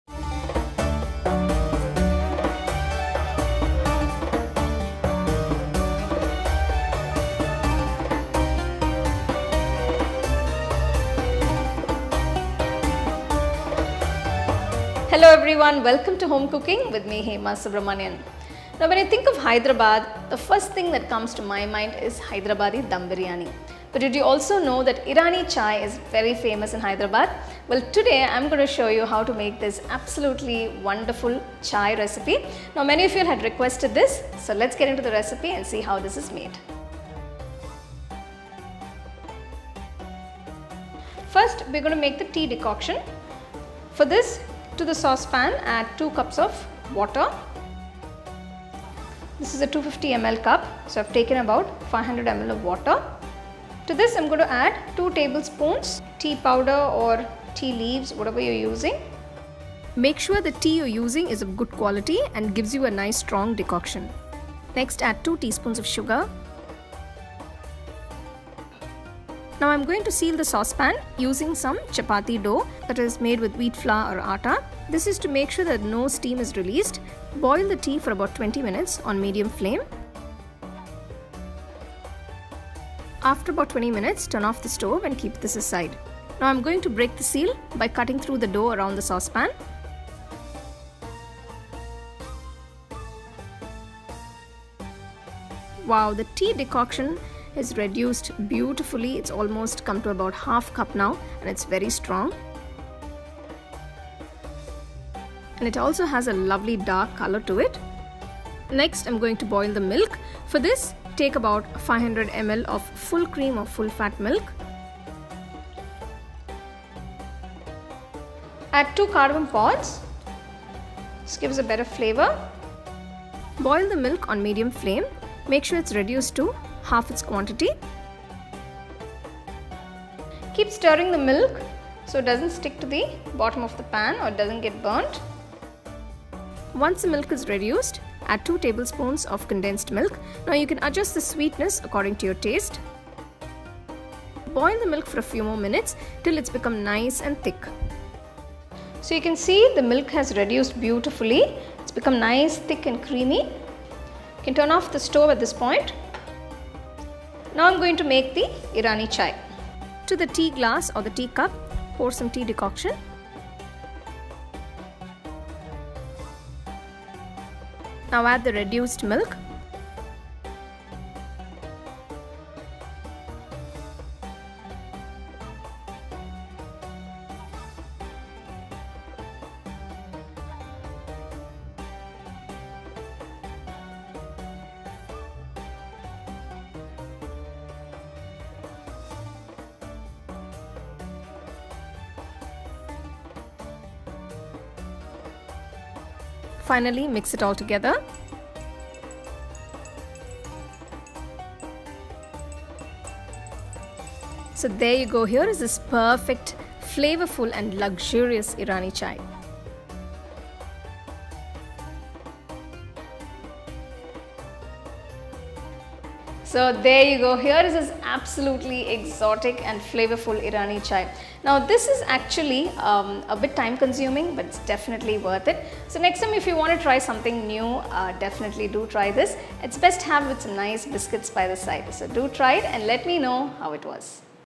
Irani Chai | Hyderabadi Irani Chai | Dum Tea Recipe
Irani Chai

Ingredients

Tea Powder - 2 Tbsp
Full Fat Milk - 500 Ml (Buy
Sugar - 2 Tsp
Condensed Milk - 2 Tbsp (Buy
Cardamom Pod - 2 Nos (Buy

Method:
1. Pour 500 ml of water to a saucepan.
2. Add tea powder, sugar and boil it.
3. To avoid any steam from escaping, seal the pot with some chapati dough.
4. Boil this for 20 mins until the tea decoction is reduced to half.
5. In another pour the milk and boil it.
6. Add cardamom pods and boil until it reduces.
7. Now add condensed milk and mix it.
8. To assemble the Irani tea, pour the tea decoction upto 1/4 of the cup.
9. Top this with the reduced milk.
10. The famous hyderabadi Irani Chai is ready.